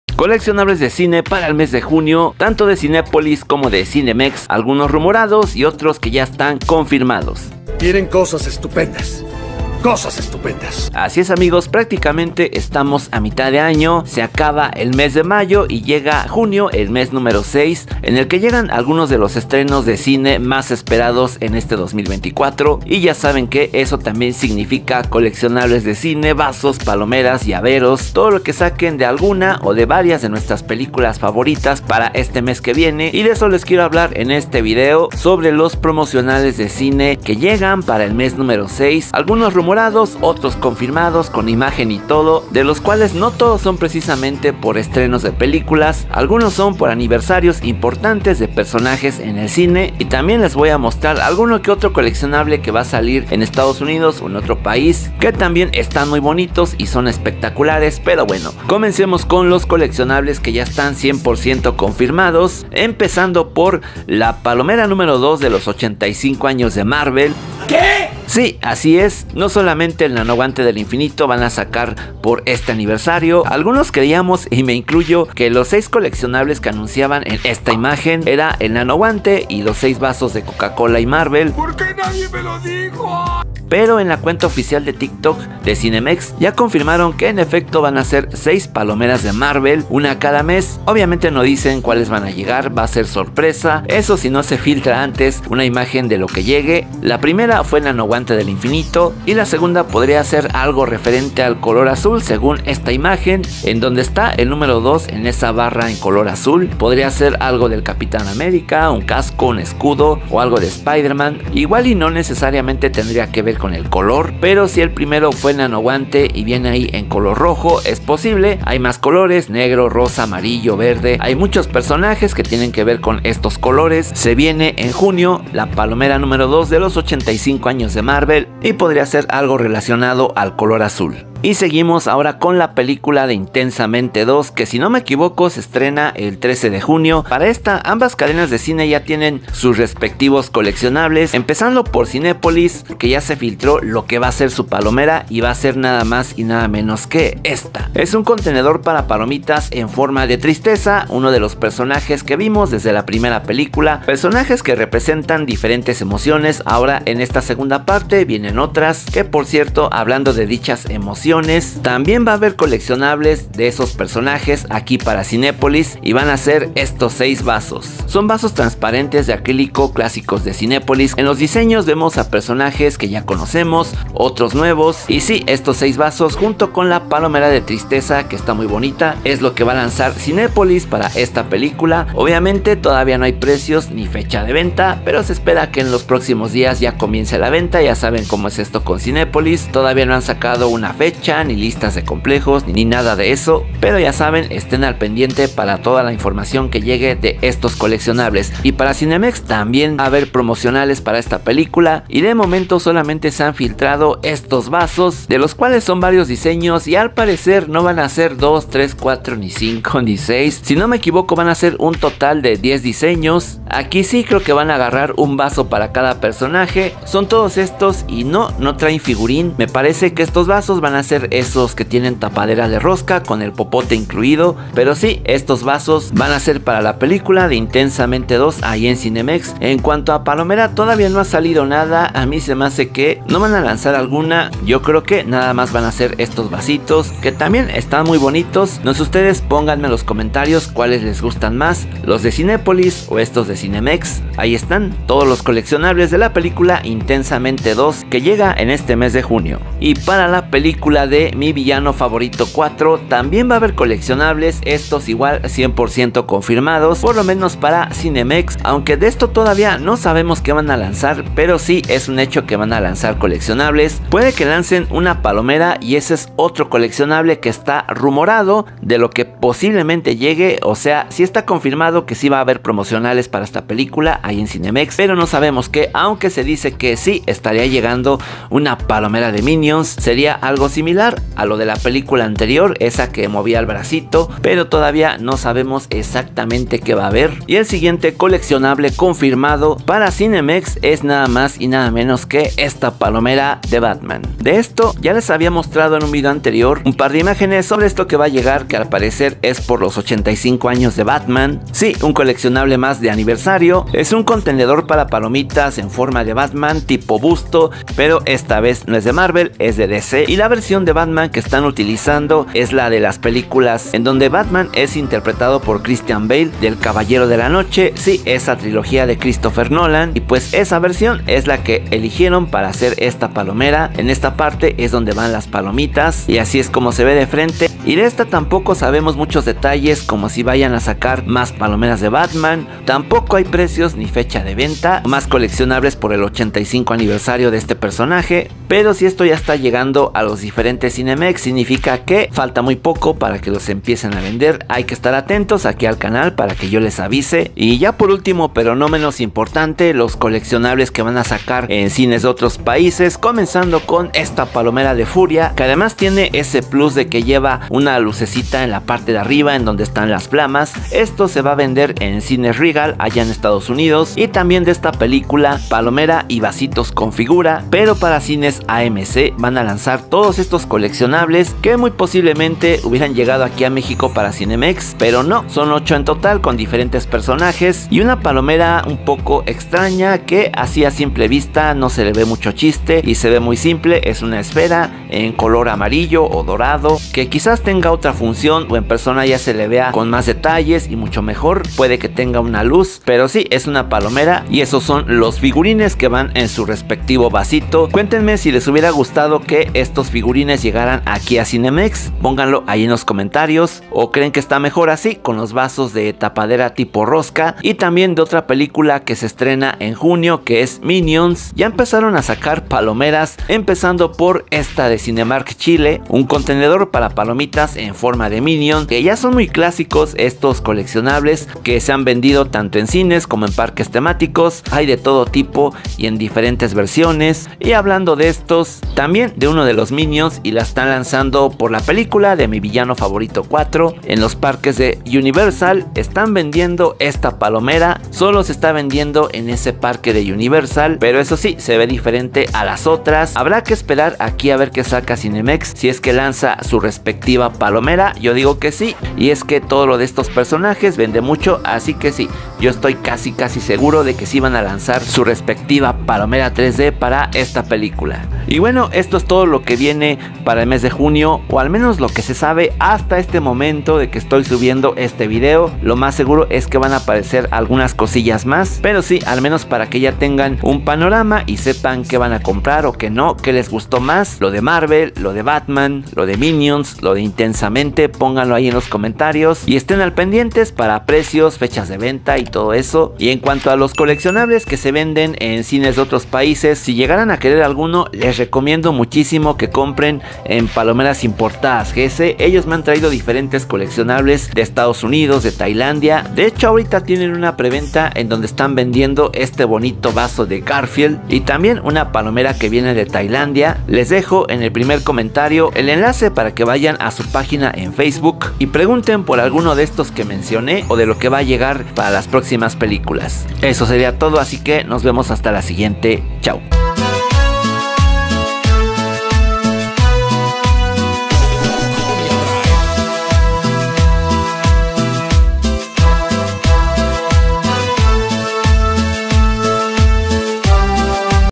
Palomeras CINEMEX y CINÉPOLIS confirmadas en JUNIO! Batman, Marvel, Intensamente 2.
Estos son los coleccionables de cine que se venderan en el mes de junio, tanto de cinepolis como de cinemex, de las películas MI VILLANO FAVORITO, INTENSAMENTE 2, y aniversarios de MARVEL y BATMAN.